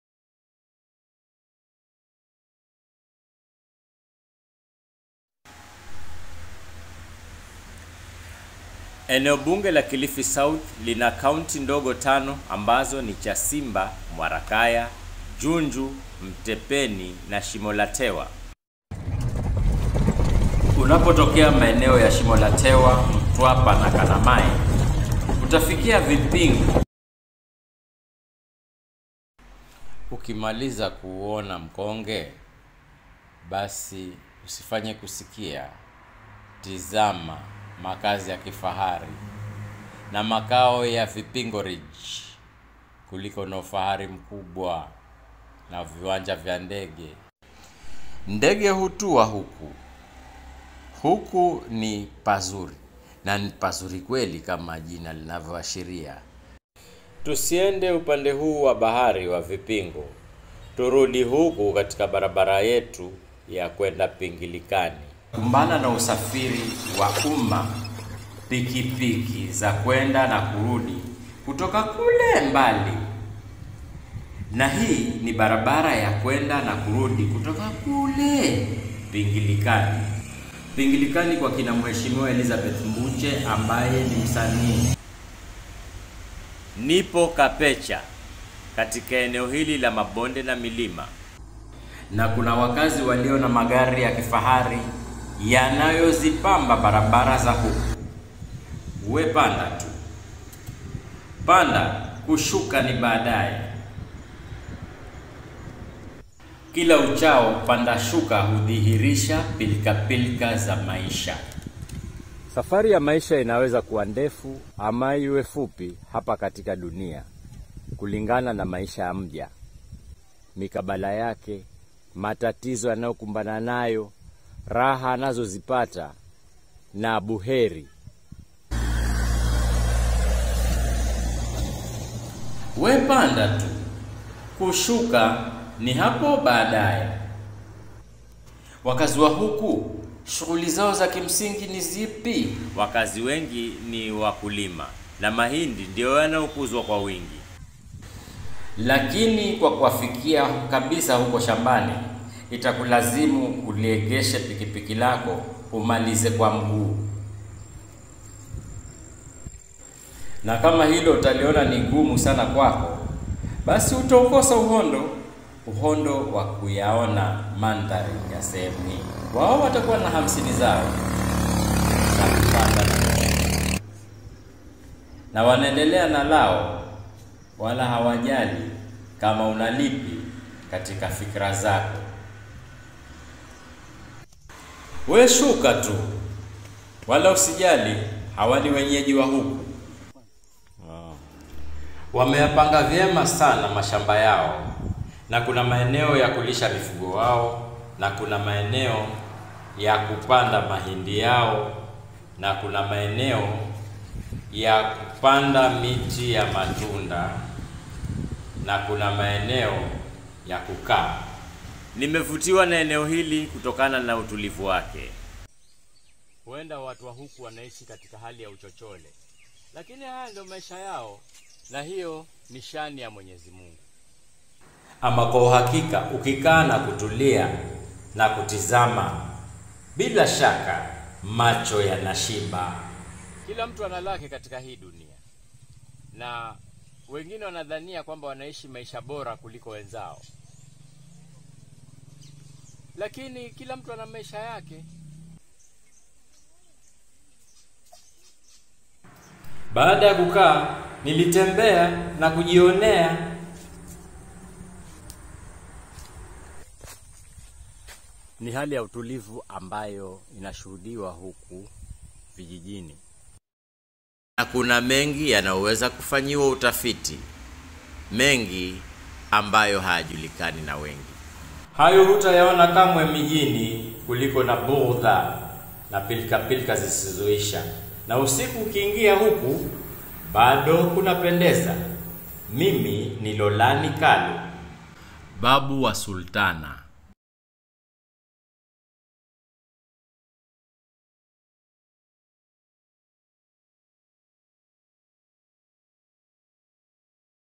Mashambani Kilifi
Hali ya utulivu katika eneo bunge la Kilifi South. Taarifa ya Lolani Kalu, Mwanahabari wa kujitegemea.